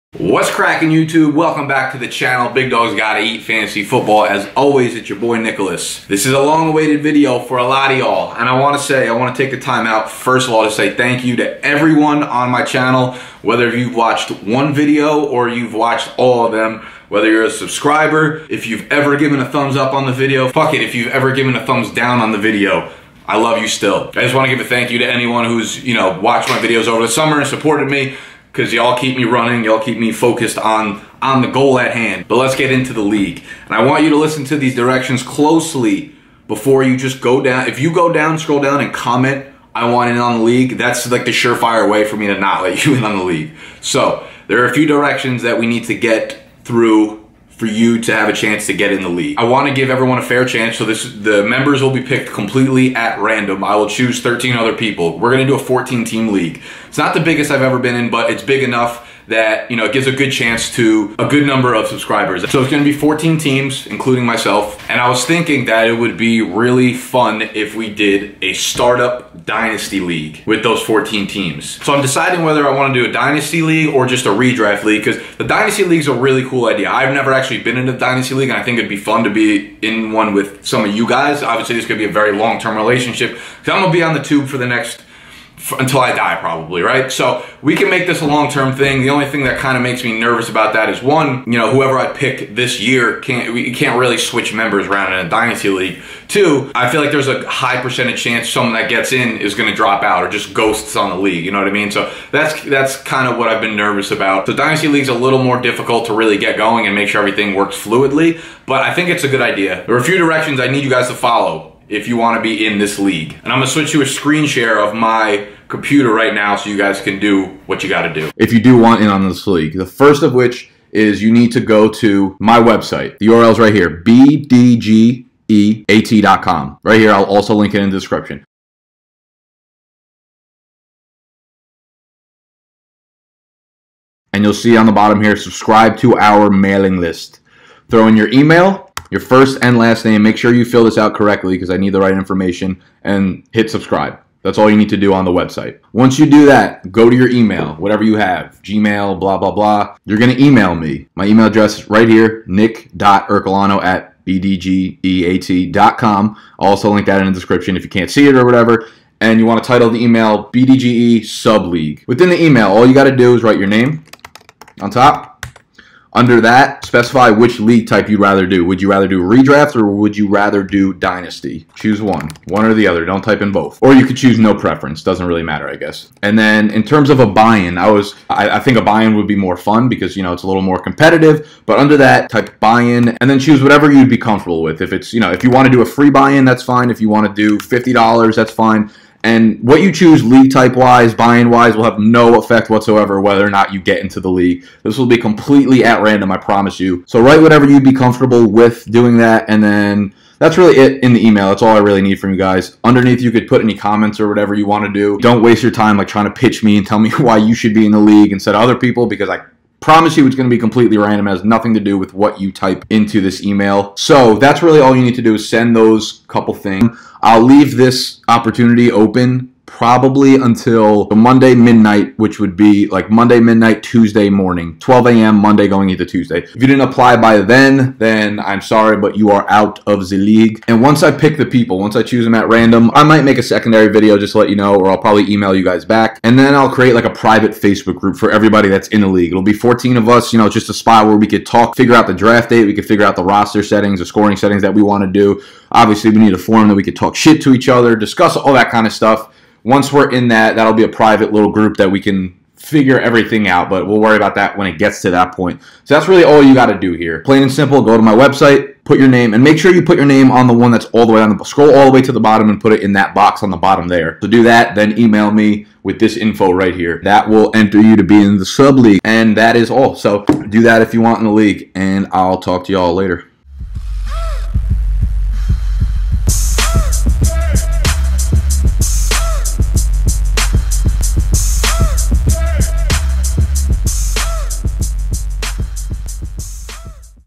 BDGE Subscribers League Open!!! - How to Join
The BDGE Sub League is officially open for applications my friends!!!
Here's what you gotta do to get in:

1.
4. In the email type your first and last name. Then choose whether you'd rather do a redraft league or a dynasty league. Then choose whether you'd want it to be a free entrance league or a $$ buy-in, and if so, what you'd be comfortable buying in for.
5. Leave any random comments at the bottom of the email if you'd like. Don't waste your time explaining why you should be in the league, it'll make no difference as to you getting in or not.

That's it. I will be choosing 13 subscribers to be in this league, completely at random. What you choose for league type and buy-in will have no affect on whether or not you get in the league.

Entry will close Monday (8/14) at midnight (going into Tuesday)./